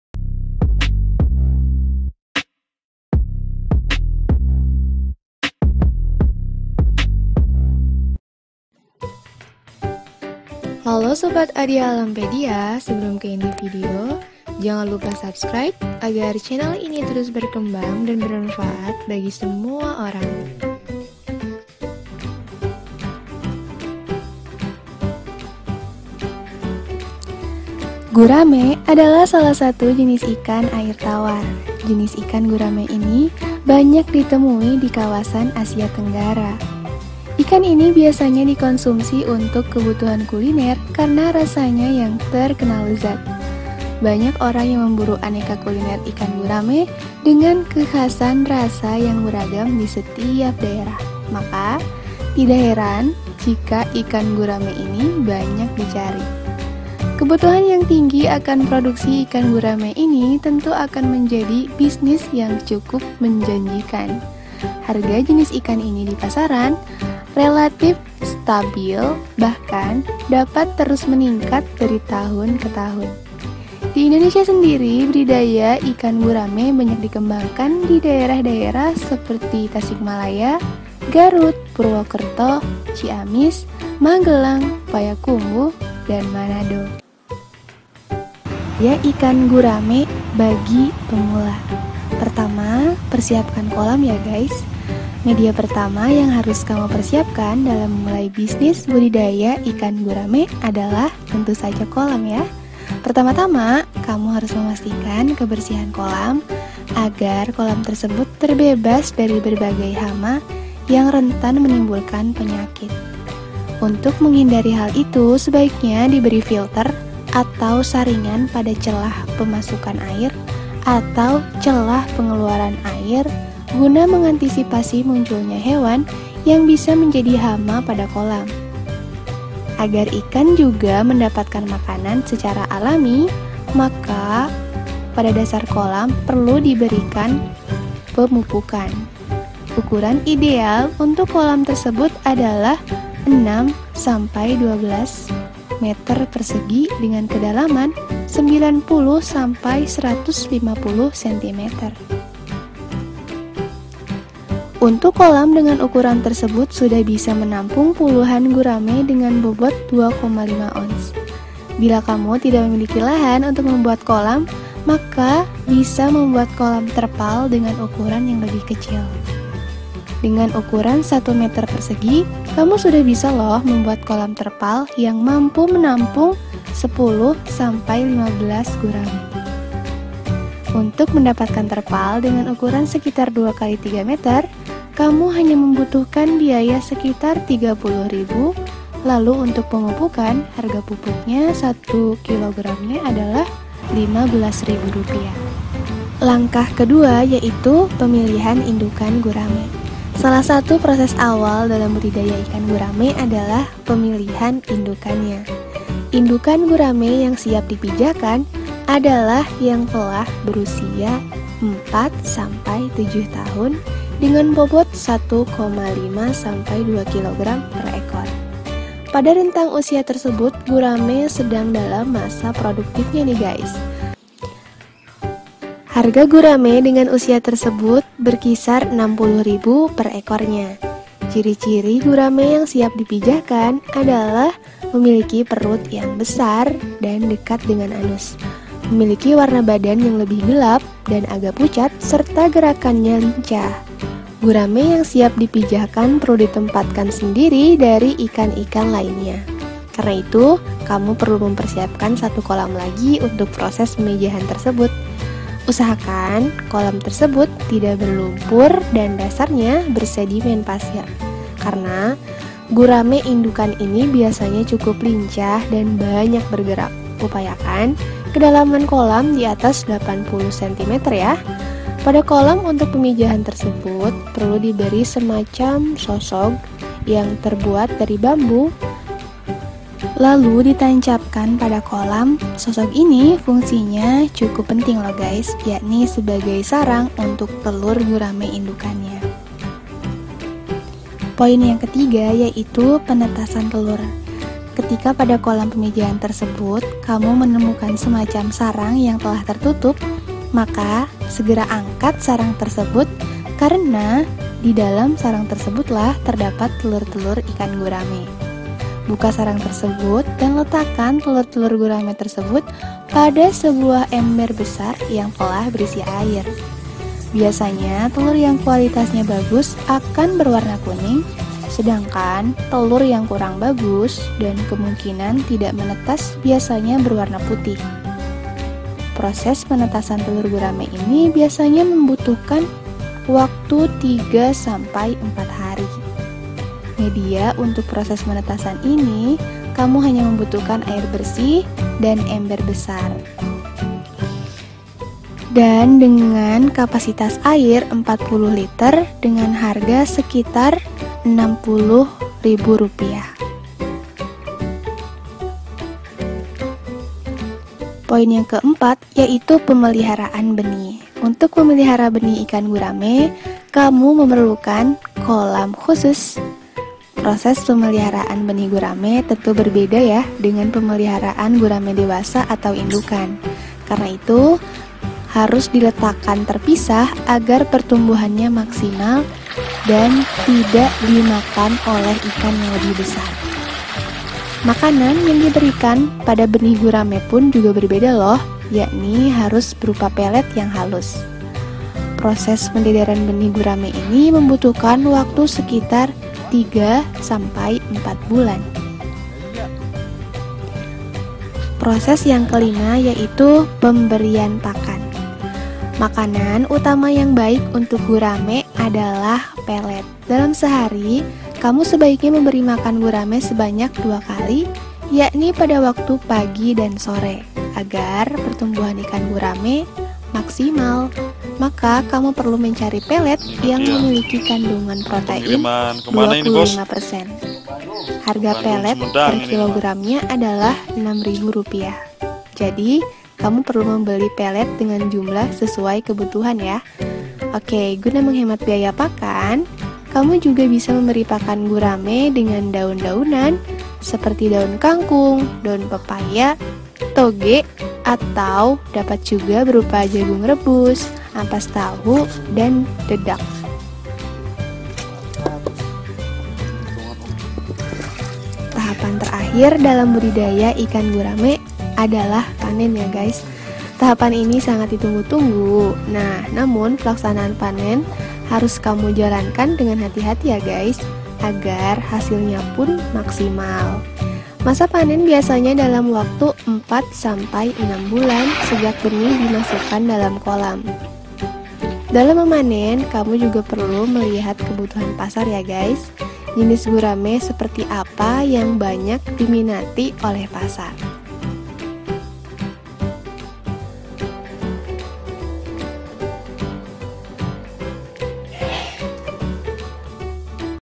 5 Cara Budidaya Ikan Gurame bagi pemula || inspirasi bisnis yang menjanjikan
Siapa sangka, ikan gurame bisa menjadikan seorang pebisnis pemula jadi jutawan! Namun, tentu ada panduan yang harus dilakukan oleh para penggiat budidaya ikan gurame agar dapat mendulang kesuksesan.

Mau tahu apa saja langkah-langkahnya? Simak dalam video  diatas ini ya guys.